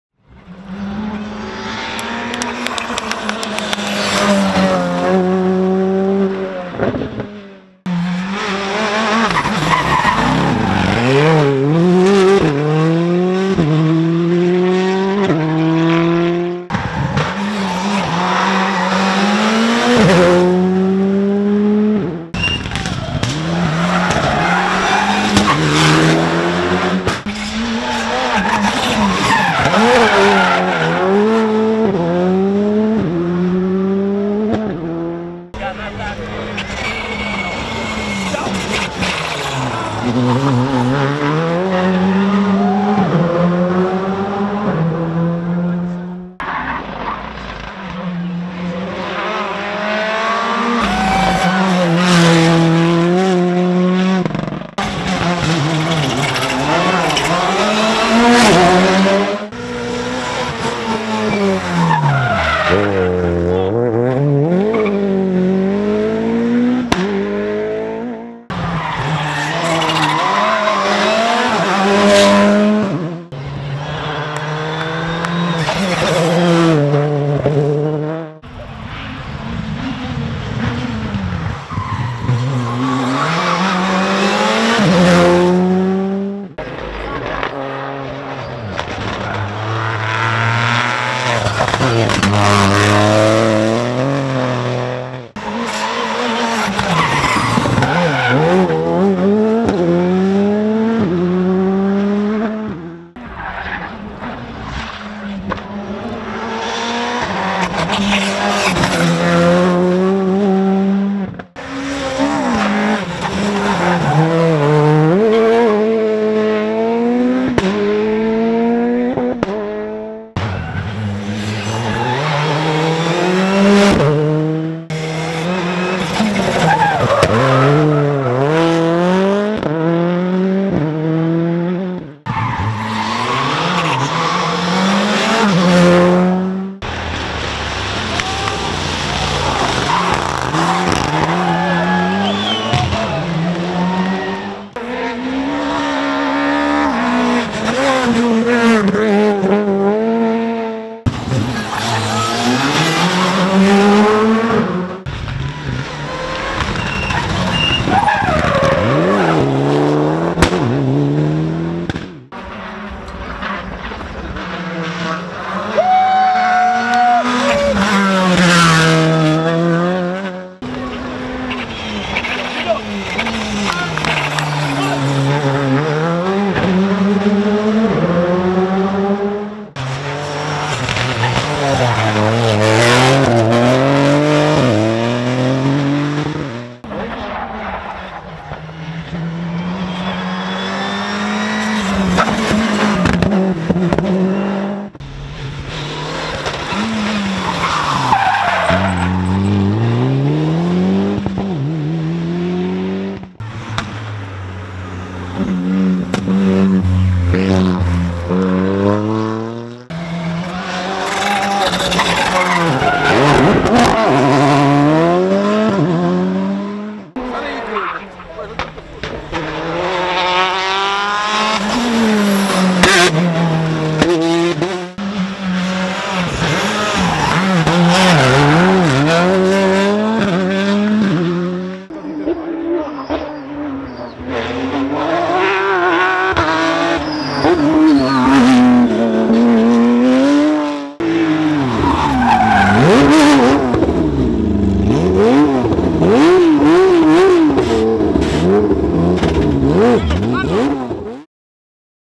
Rally Castelo Branco 2020 | CPR 2020 | WRCantabria
Resumen de la vuelta a la normalidad en el Campeonato de Portugal de Rallyes.